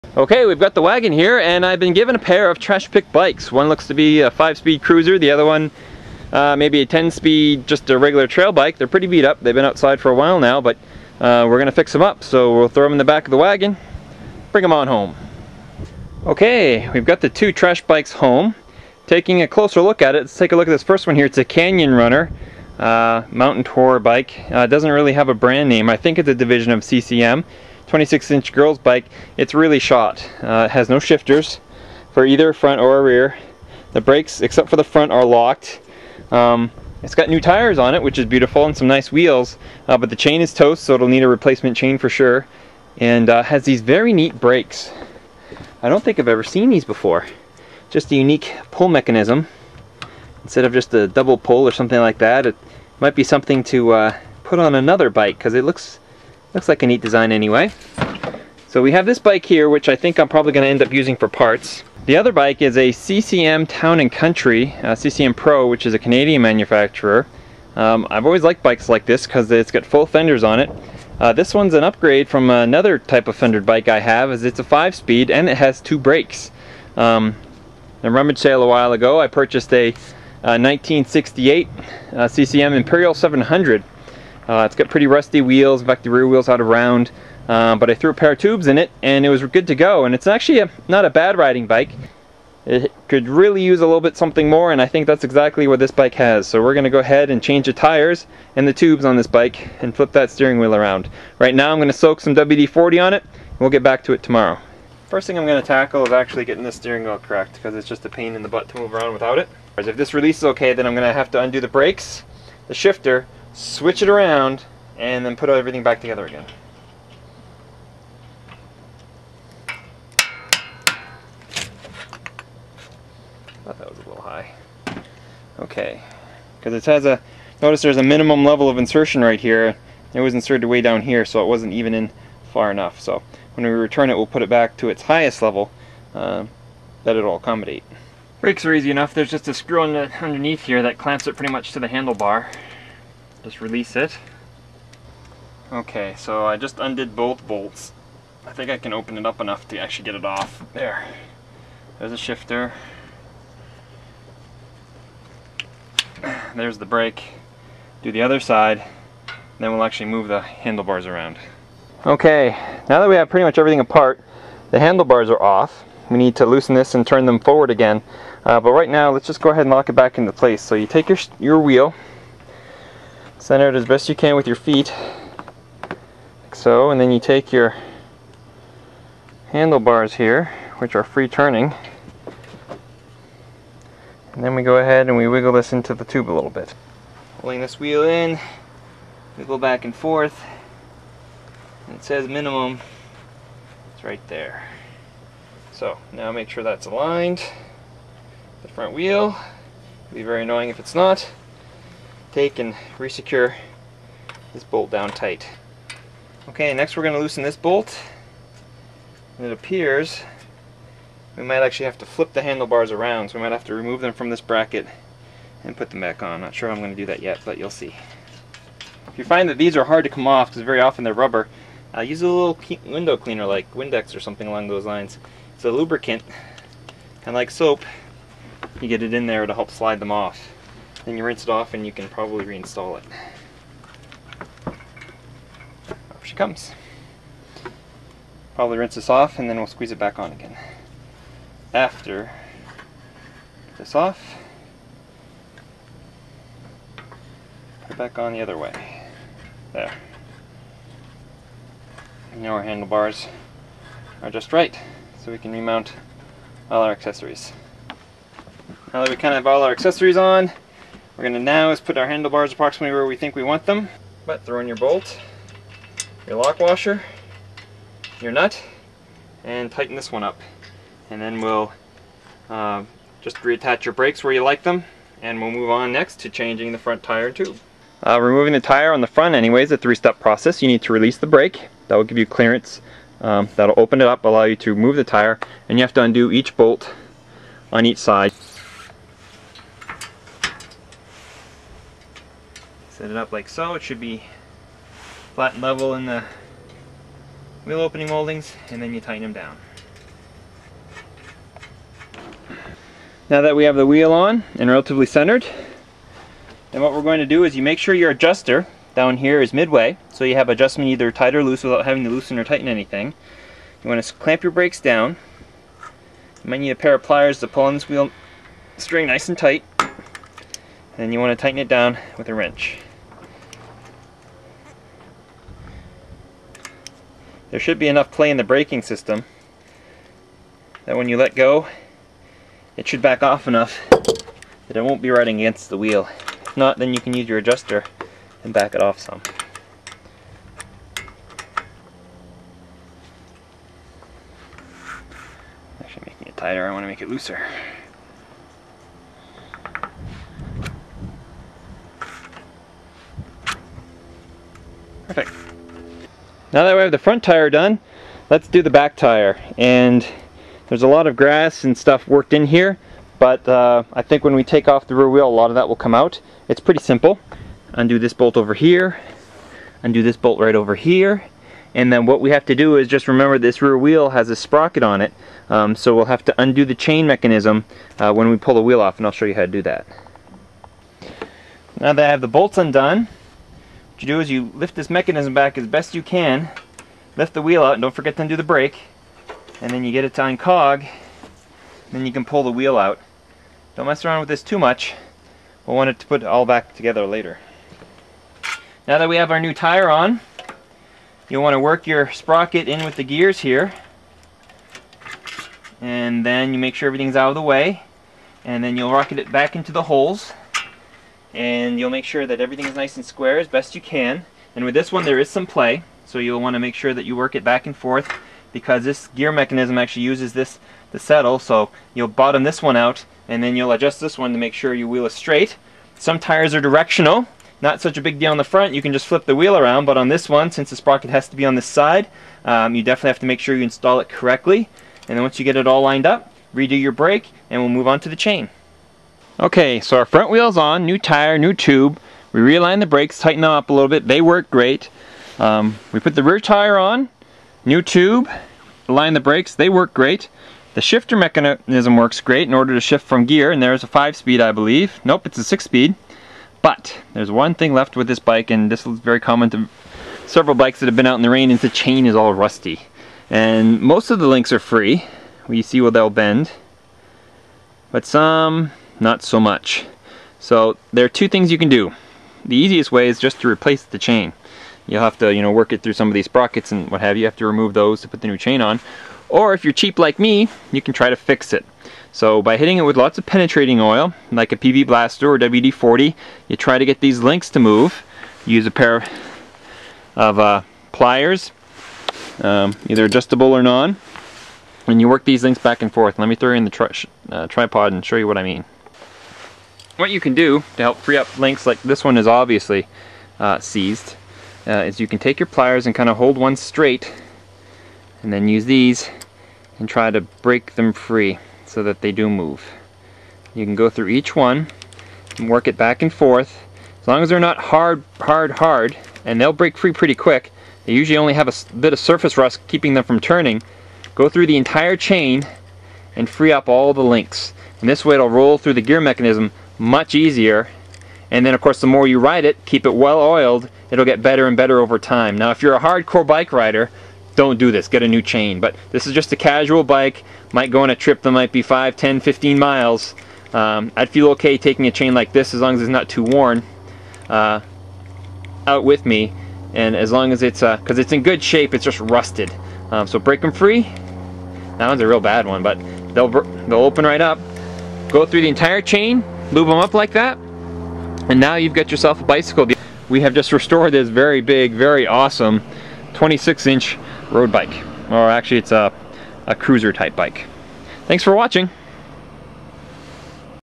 Fixing a trash picked bicycle! She's a beauty with fenders!
In this video I walk through the steps of fixing a trash picked cruising bike.  Work that needed to be done included switching the handle bars back to the correct orientation, replacing both tires and tubes, some chain work, and other odds and ends.

Sucks not having a garage to play in, but it passes the time.  Had a hard time finding parts for it this late in the season.